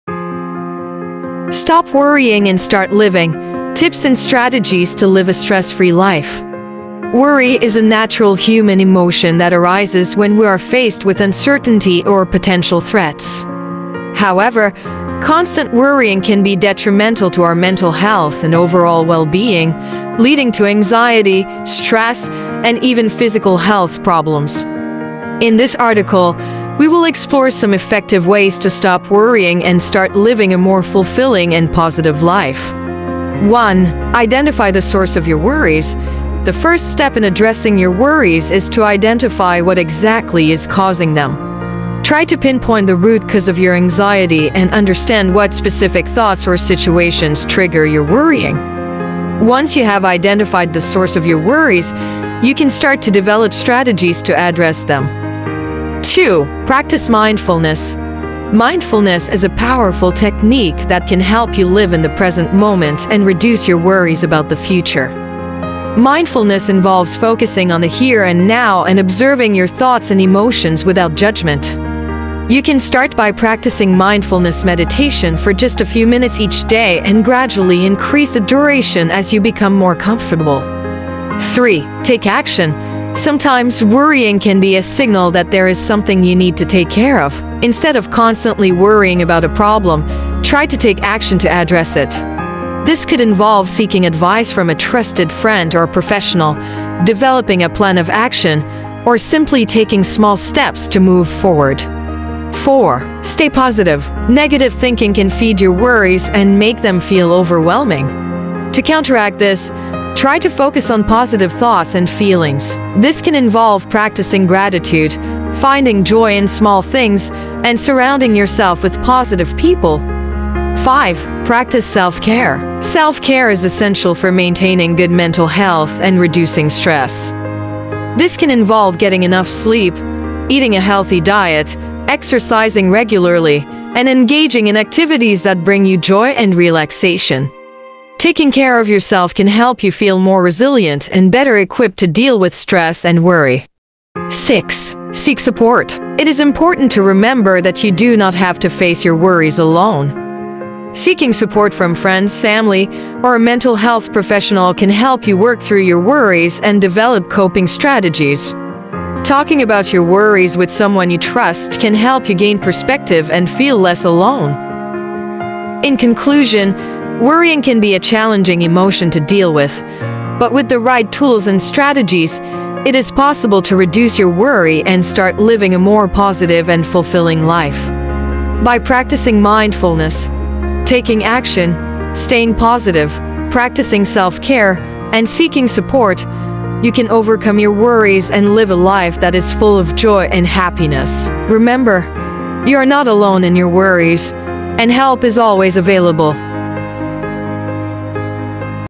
Stop Worrying & Start Living: Tips and Strategies to Live a Stress-Free Life
In this video, we explore the different ways you can stop worrying and start living a stress-free life. We cover various tips and strategies that can help you deal with stress and anxiety. Whether you're dealing with work-related stress or personal problems, we provide practical advice to help you manage your worries and lead a happier life. From mindfulness practices to healthy habits, we cover it all in this video.